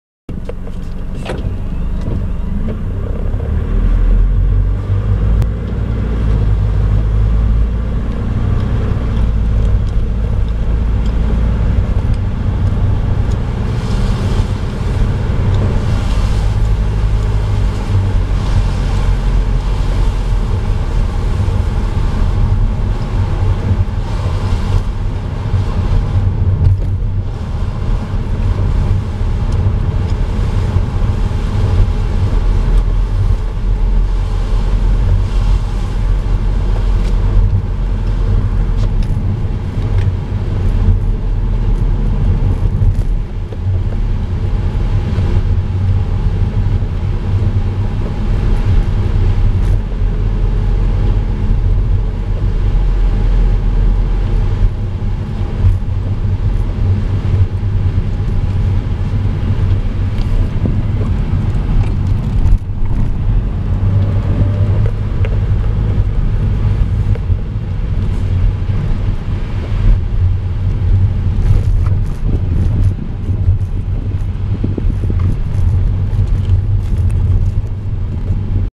NV350キャラバン クリーンディーゼルエンジン 5AT変速の様子 郊外走行
NV350(E26/YD25DDTi/2WD/標準ボディ)の郊外走行の様子。